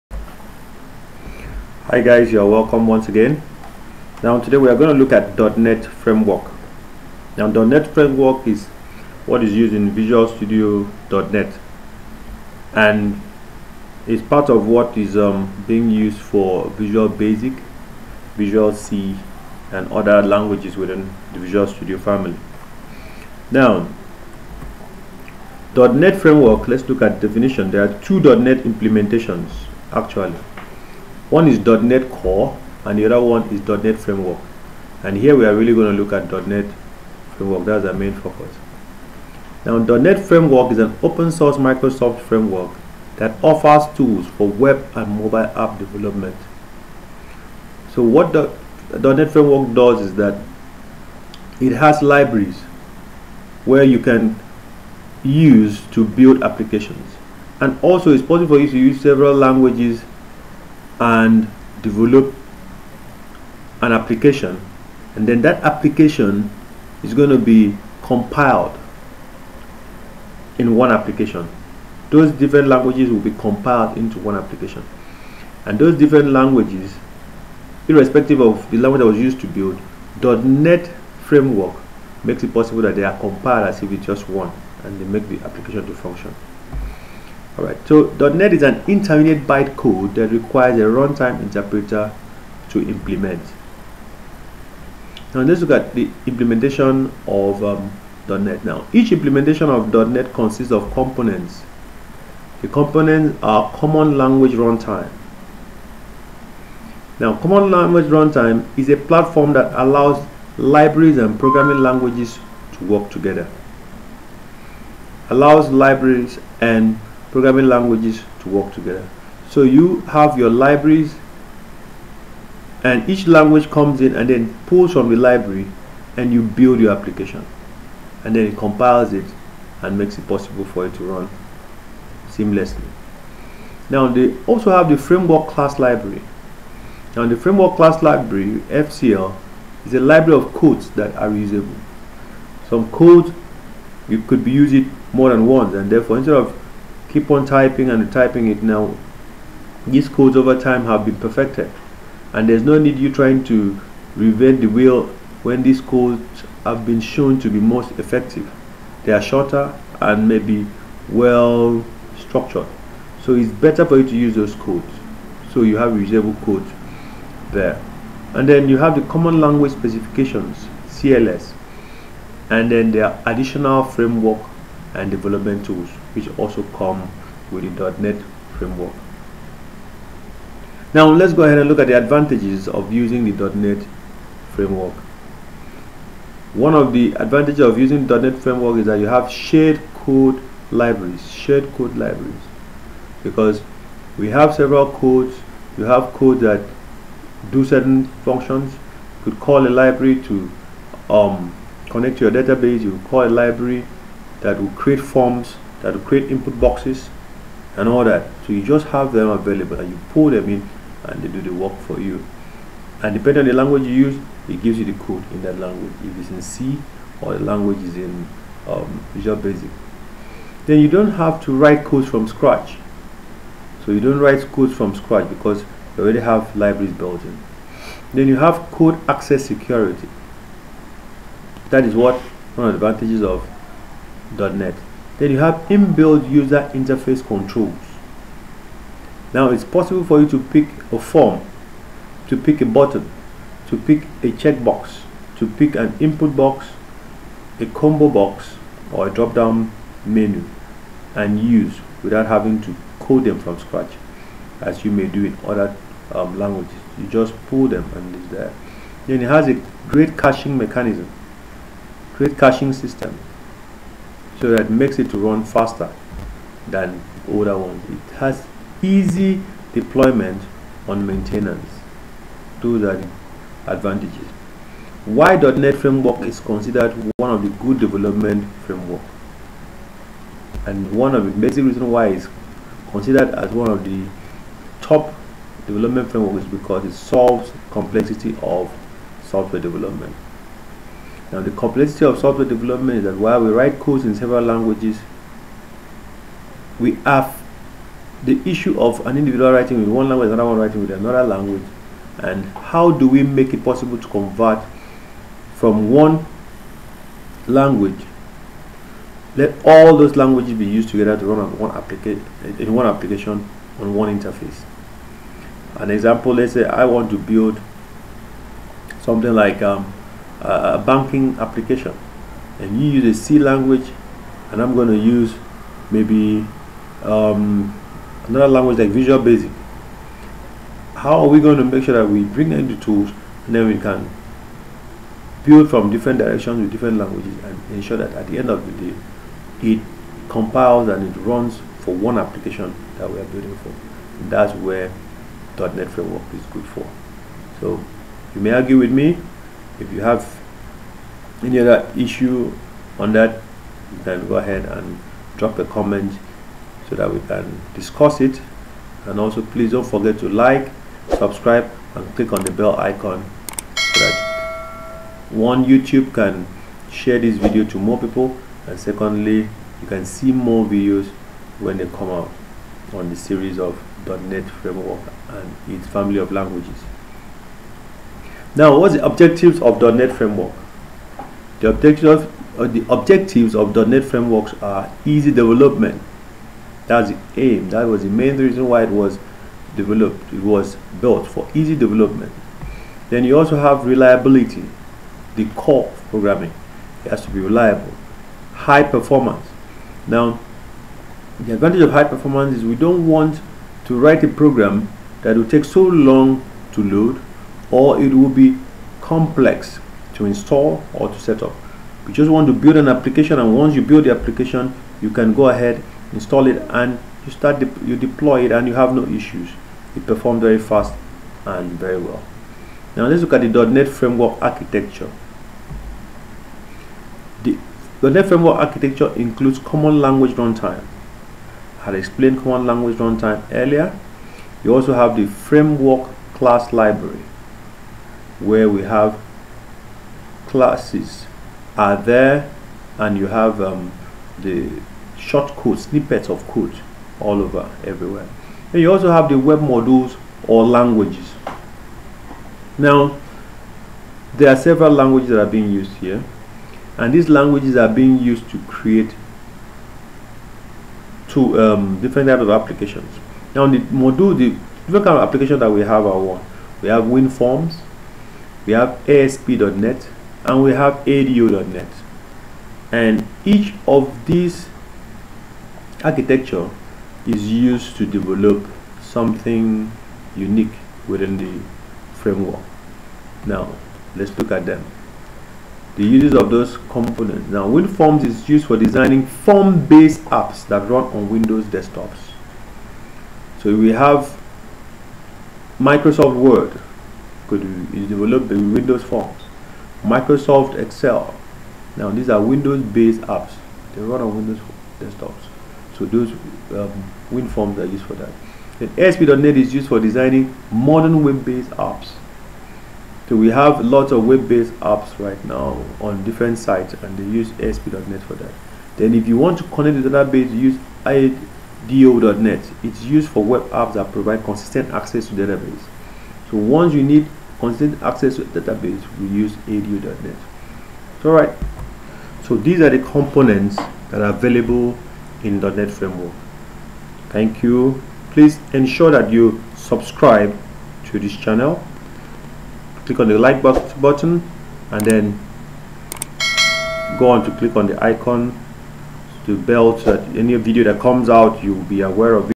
.NET FRAMEWORK | COMPLETE GUIDE
In this video, we’ll cover the major features of the .NET Framework as well as its architecture components, and benefits.

0:00 Introduction
1:34 EACH IMPLEMENTATION OF .NET CONSIST OF COMPONENTS
2:59 ADVANTAGES OF USING THE .NET FRAMEWORK
4:50 WHY .NET FRAMEWORK IS CONSIDERED ONE OF THE GOOD DEVELOPMENT FRAMEWORK
6:17 OBJECTIVES OF .NET FRAMEWORKS
7:02 NET FRAMEWORK ARCHITECTURE
8:21 USES OF THE COMPONENTS